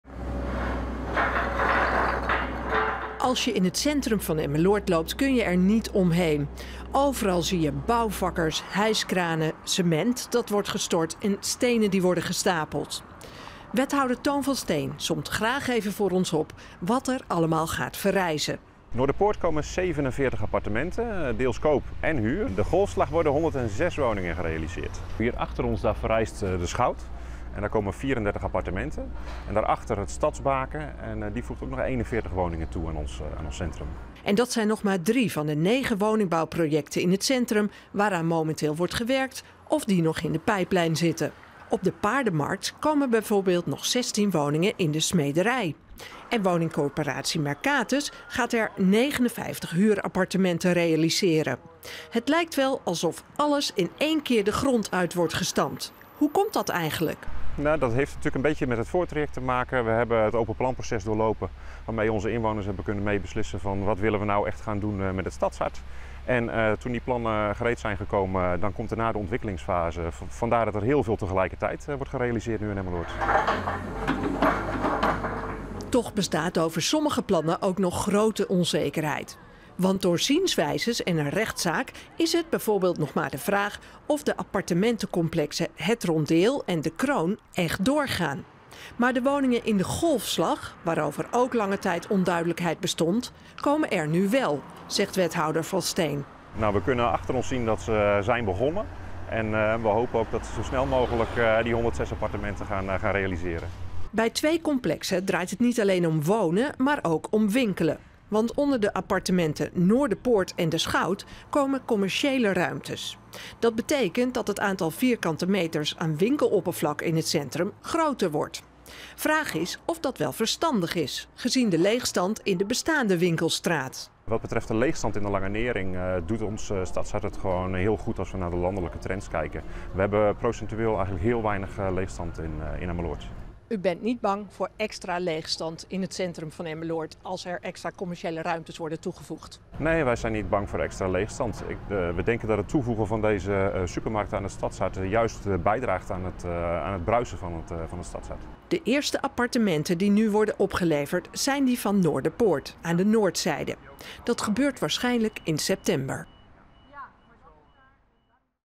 Honderden nieuwe woningen in centrum van Emmeloord
Het centrum van Emmeloord krijgt er de komende tijd honderden nieuwe appartementen bij. Op dit moment wordt op verschillende plekken druk gebouwd  bijvoorbeeld bij de Golfslag  de Paardenmarkt en winkelstraat de Lange Nering. In totaal wordt er gewerkt aan negen woningbouwprojecten. De eerste huizen die worden opgeleverd zijn die van appartementencomplex Noorderpoort aan de Noordzijde. In september kunnen de bewoners daar intrekken.